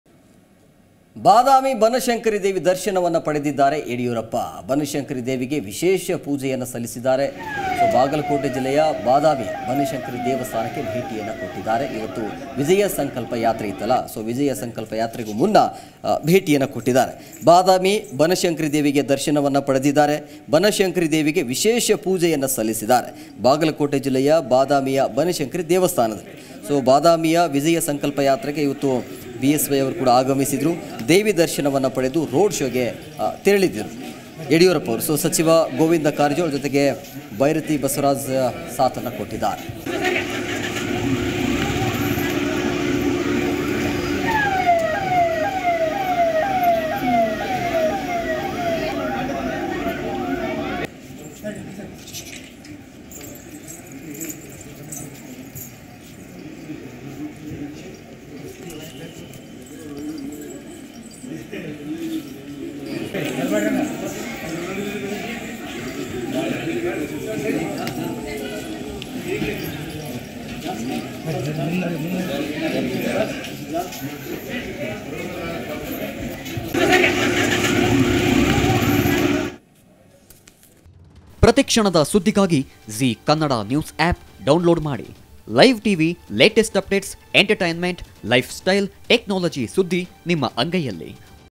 Banashankari Devi | Badami | BS Yediyurappa | ಬನಶಂಕರಿ ದೇವಿಗೆ BSY ವಿಶೇಷ ಅರ್ಚನೆ
ಯಡಿಯೂರಪ್ಪ ಬಾಗಲಕೋಟೆ ಜಿಲ್ಲೆಯ ಬಾದಾಮಿ ಬನಶಂಕರಿದೇವಿ ದರ್ಶನ ಪಡೆದಿದ್ದಾರೆ.. ಬನಶಂಕರಿ ದೇವಿಗೆ ವಿಶೇಷ ಅರ್ಚನೆ ಸಲ್ಲಿಸಿದ್ದಾರೆ..